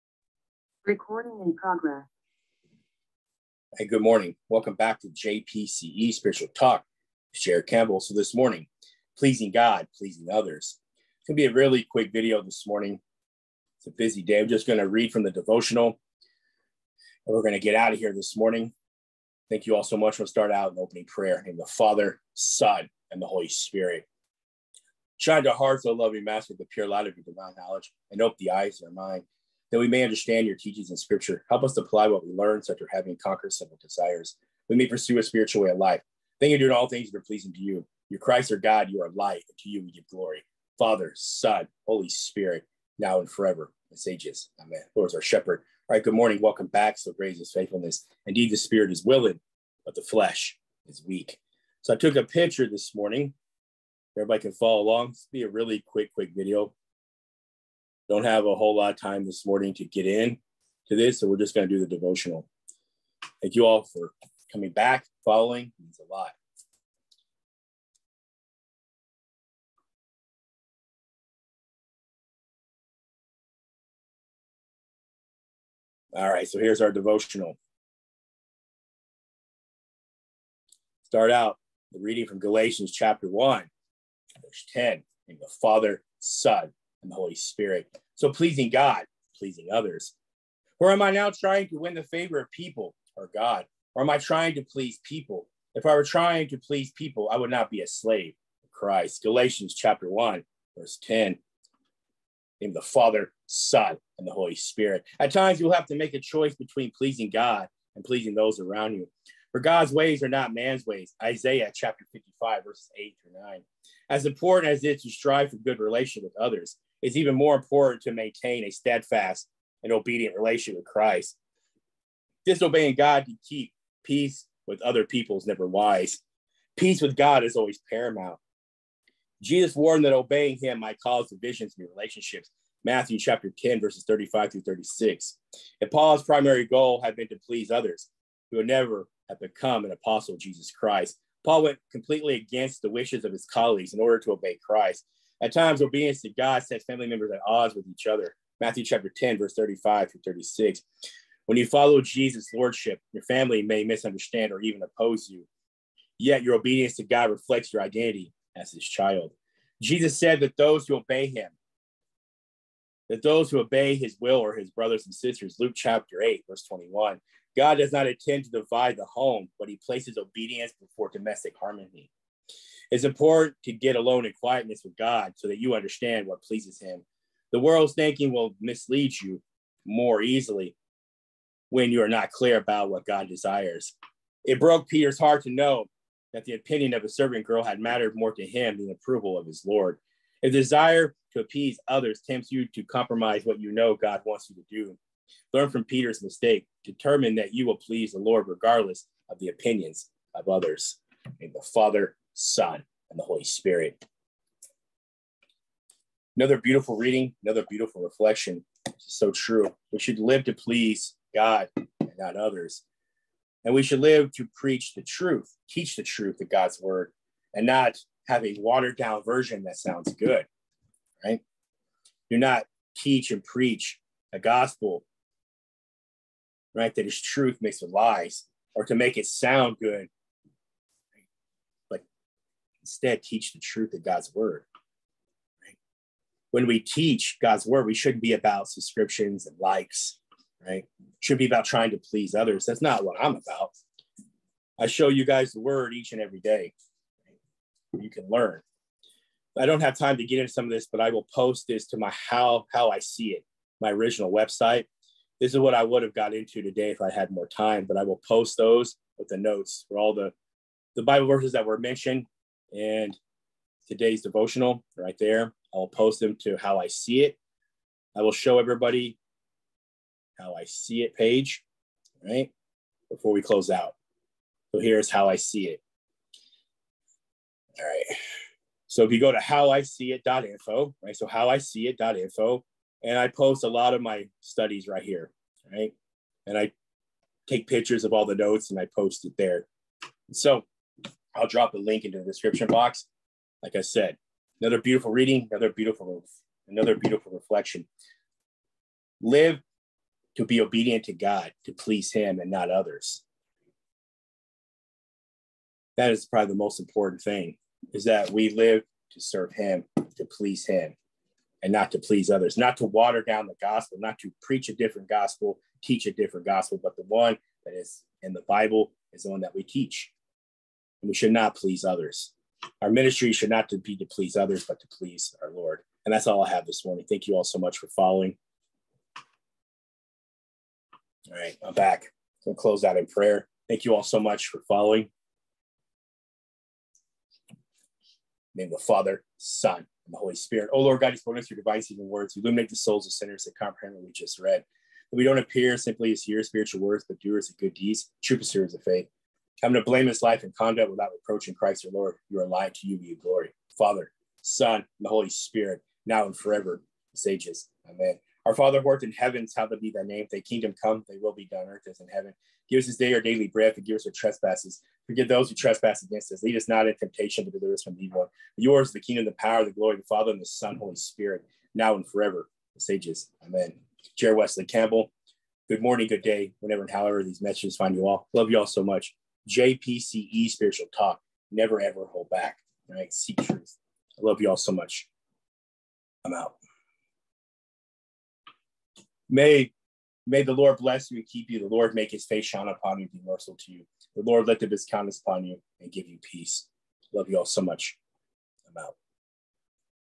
DAY 47: "PLEASING THE LORD OR PLEASING OTHERS"- See notes for links mentioned in the Devotional.
DAY 47: "PLEASING THE LORD OR PLEASING OTHERS"

Galatians 1:10
 For do I now persuade men, or God? Or do I seek to please men? For if I still pleased men, I would not be a bondservant of Christ.

Numbers 6:24-26
25 The Lord make His face shine upon you,
26 The Lord [a]lift up His countenance upon you,

In Christ love ❤️ Jared W Campbell